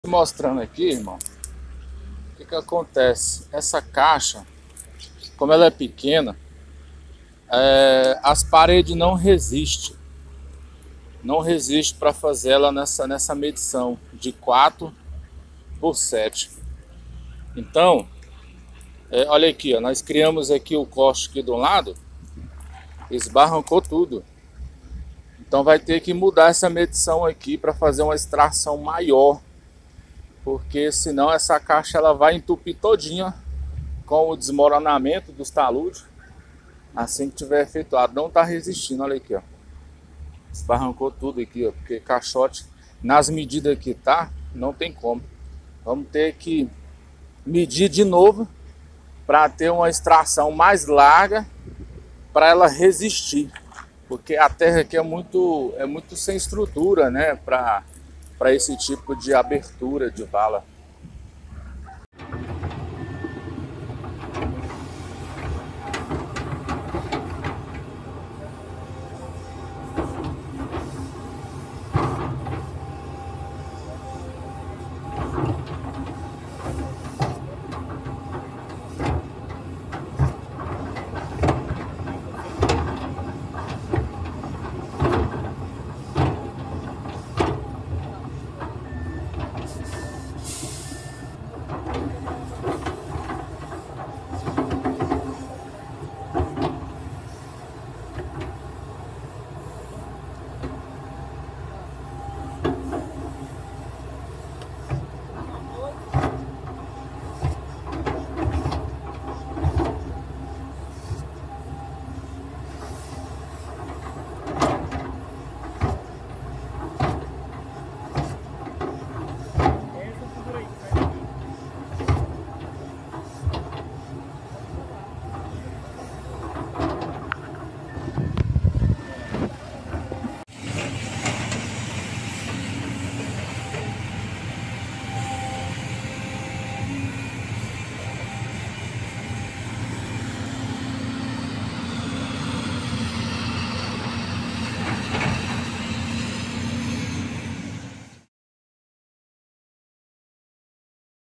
Betanhêr abrindo a caixa do sumido pluvial...# volvo 210volvo
Ariel Betanhêr  finalizando caixa pluvial do residencial pietra!